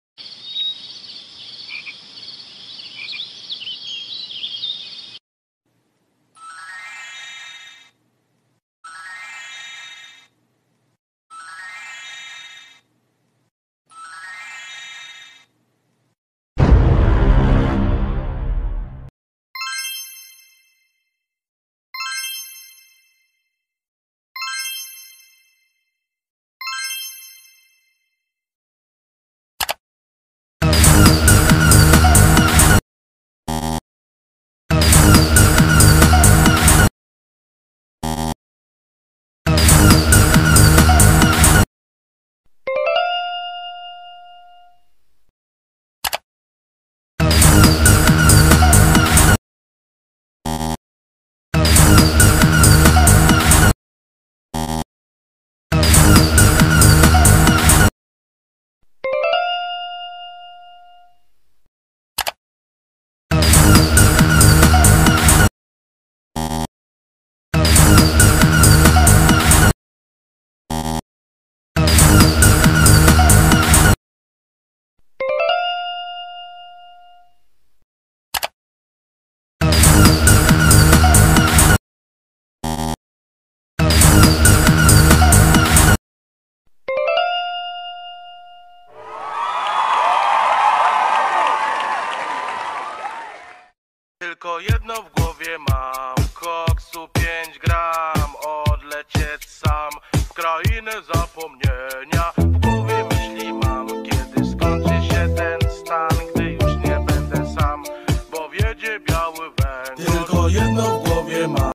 Puzzle of CUTE ANIMALS, funny tiger lion rubbit and wolf 可爱的动物拼图，有趣的狼虎狮子 rubbit
Hello everyone. My name is Daniel .